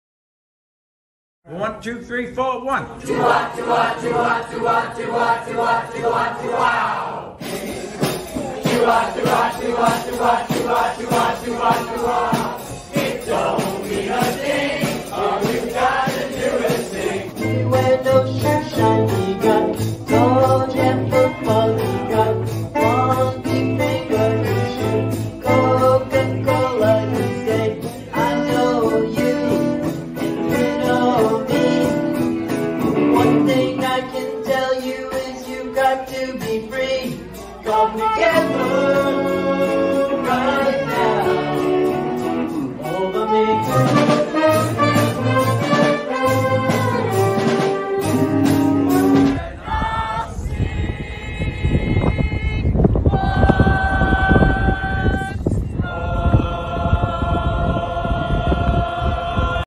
FA Music in the Mountains 2022
Falmouth Academy's annual summer student musical tour, Music in the Mountains, returned this June with 5 days/4 nights of touring and performing in NH.